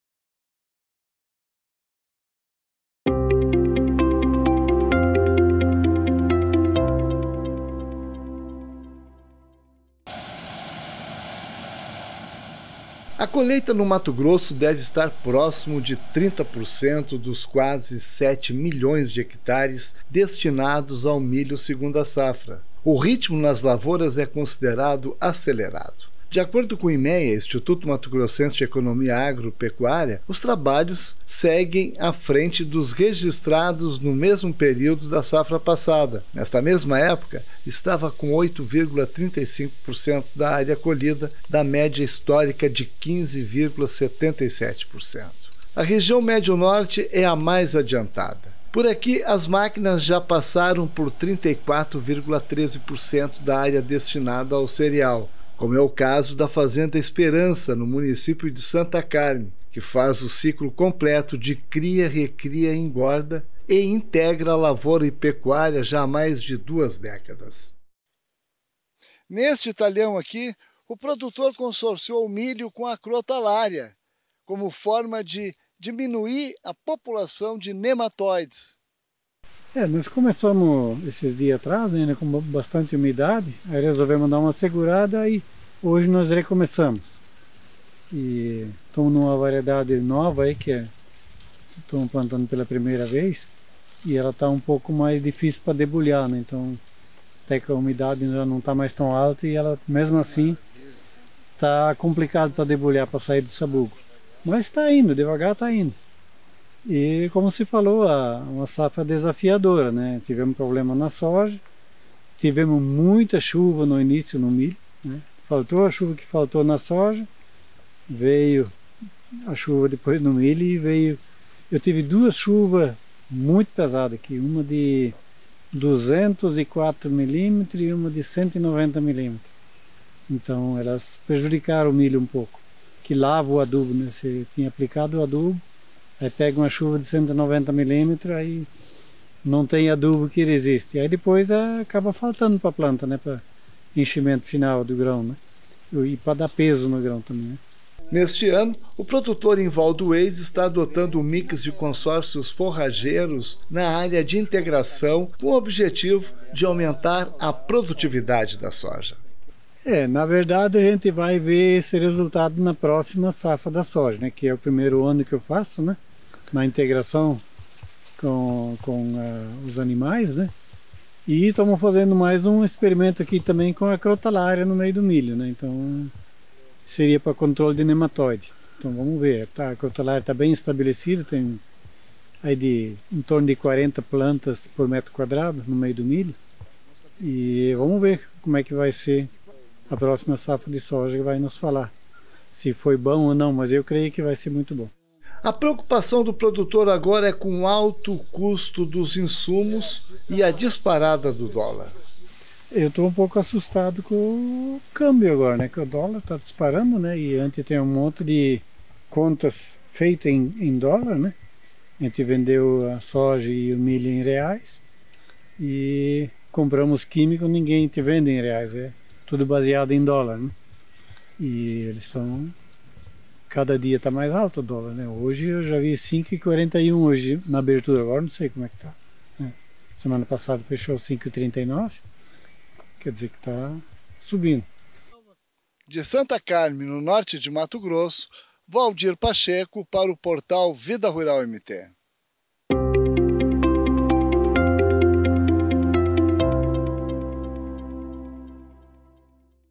COLHEITA AVANÇA E A PREOCUPAÇÃO AGORA É COM O CÂMBIO
EM ALGUMAS REGIÕES DE MATO GROSSO A COLHEITA DO MILHO SEGUNDA SAFRA ESTÁ BEM ACELERADA,  MAS A PREOCUPAÇÃO DO PRODUTOR É COM O DÓLAR QUE ESTÁ EM ALTA NESTES DIAS – VALDIR PACHECO TEM MAIS INFORMAÇÕES